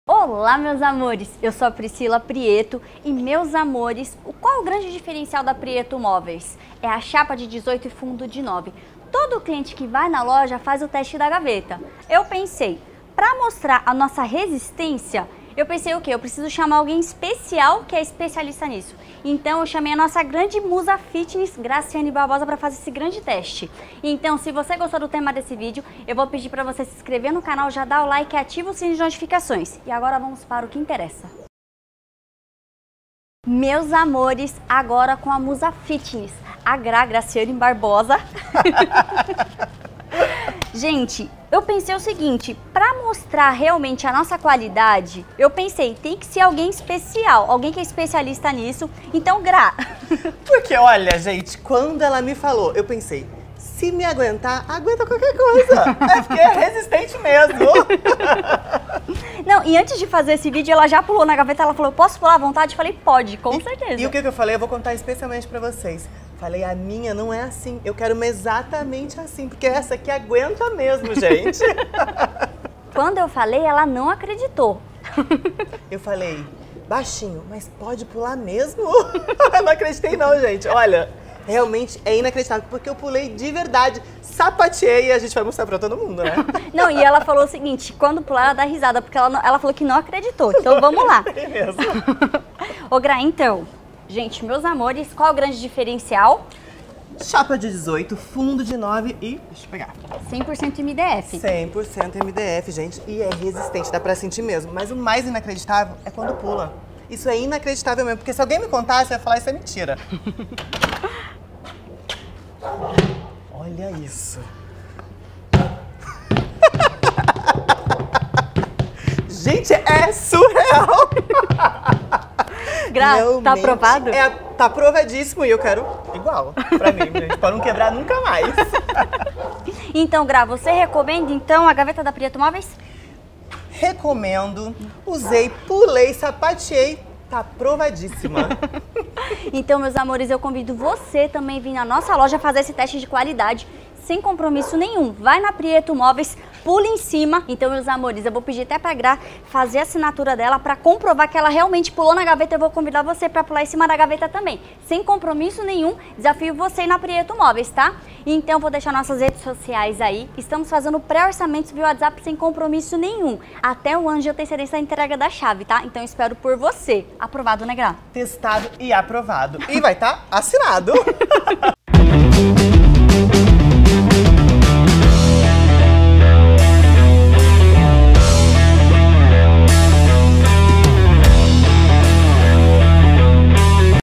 Prieto Móveis - Gracyanne Barbosa fazendo o teste da gaveta, qualidade em móveis planejados
Meus amores nesse vídeo convidamos a musa fitness Gracyanne barbosa para fazer o tão falado teste da gaveta, comprovando a qualidade da Prieto Móveis.
Trabalhamos somente com produtos 100% MDF, na cozinha só trabalhamos com chapa 18mm e fundo de 9mm.

Endereços:
Loja 01 - Av Das Esmeraldas 624 Jd Mutinga, Osasco - SP

Loja 02 - Av Benedito Alves Turibio 586 Jd. Veloso Osasco - SP